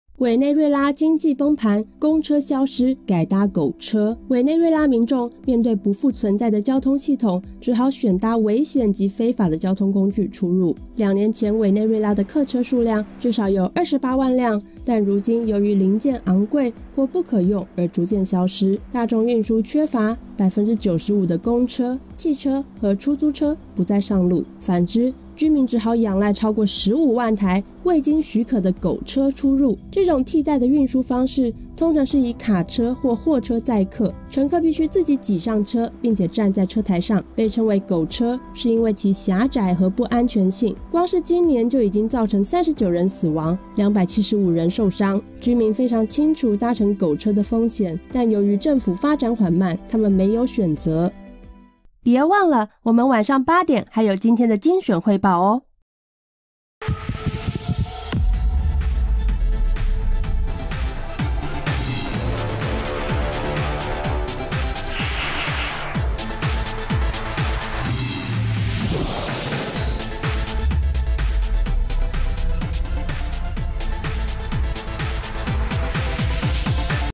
委內瑞拉經濟崩盤 公車消失改搭「狗車」
委內瑞拉民眾面對不復存在的交通系統，只好選搭危險及非法的交通工具出入。兩年前，委內瑞拉的客車數量至少有28萬輛，但如今由於零件昂貴或不可用而逐漸消失。大眾運輸缺乏，95％的公車、汽車和出租車不再上路。反之，居民只好仰賴超過15萬台未經許可的「狗車」出入。這種替代的運輸方式，通常是以卡車或貨車載客，乘客必須自己擠上車並站在車台上。被稱為「狗車」，是因其狹窄和不安全性，光是今年就已經造成39人死亡、275人受傷。居民非常清楚搭乘「狗車」的風險，但由於政府發展緩慢，他們沒有選擇。